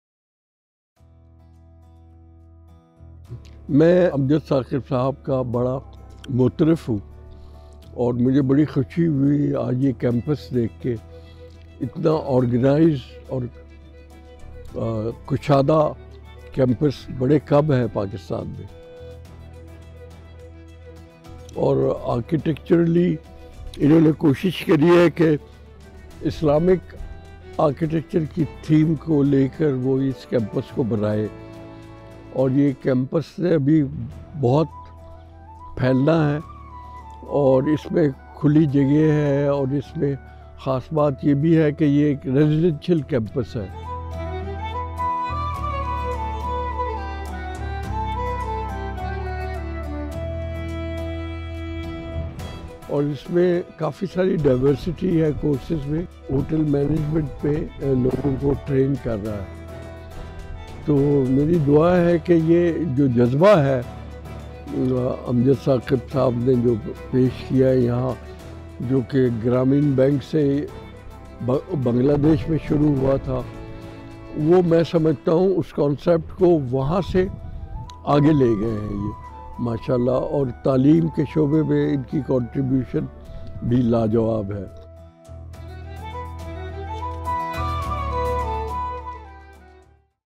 Akhuwat College, Kasur, welcomed Nayar Ali Dada, legendary and renowned Architect
Renowned architect Nayar Ali Dada shares his remarks about Akhuwat.